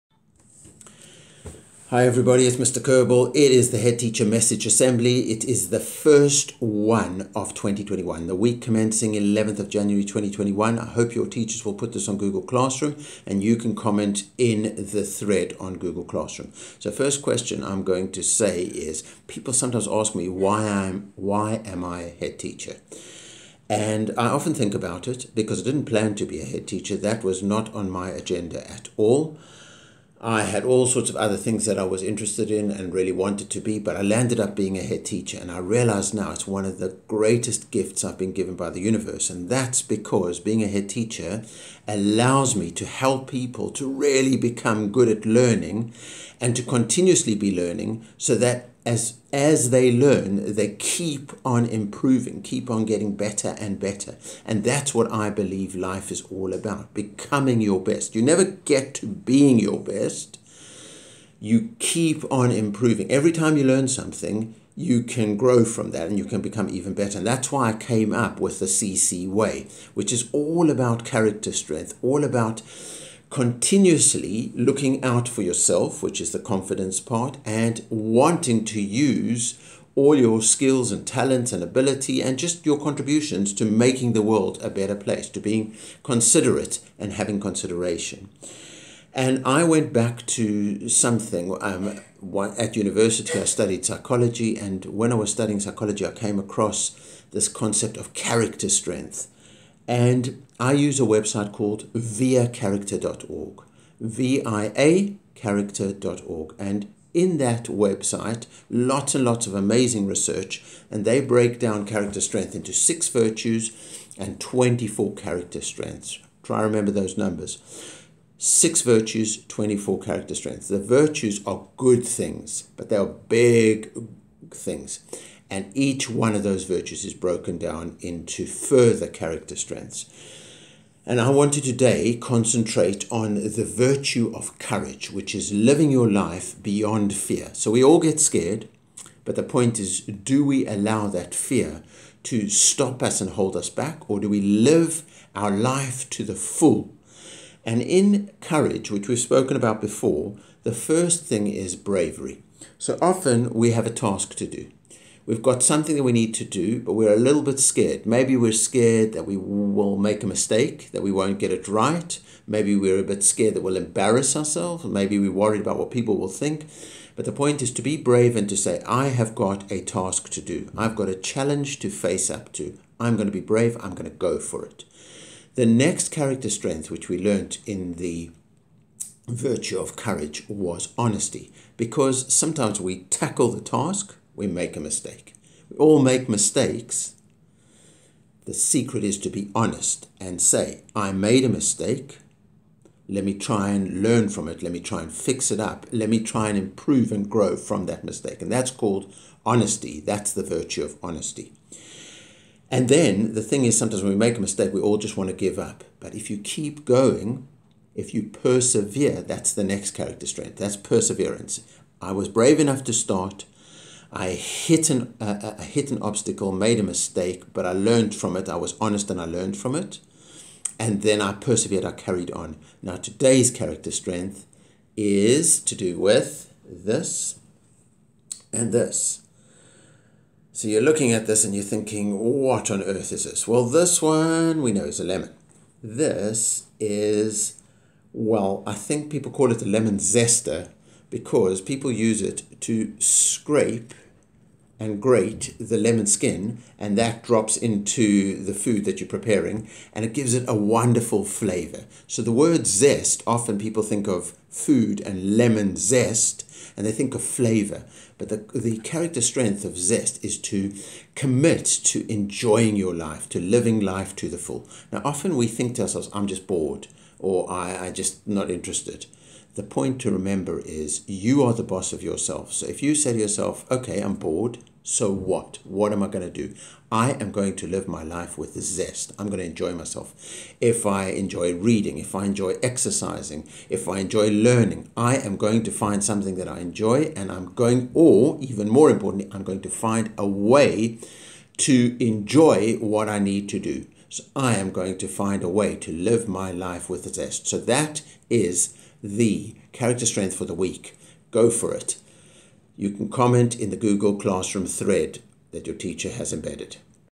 HT message assembly yr 5 and 6 Zest
introducing the character strength of Zest. Find a way to enjoy life and make the most of it = ZEST.